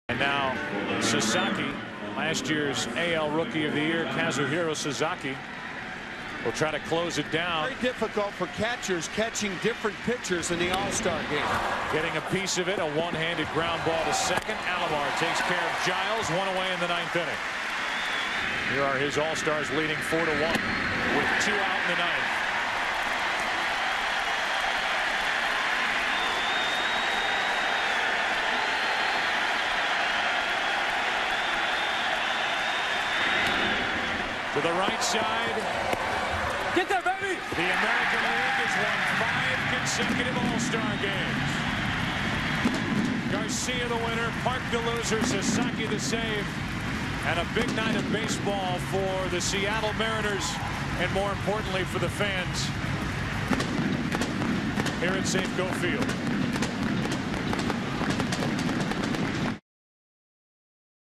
2001 ASG: Sasaki picks up the save for the American League
7/10/01: Mariners closer Kazuhiro Sasaki picks up the save for the American League in their 4-1 win over the National League in the 2001 All-Star Game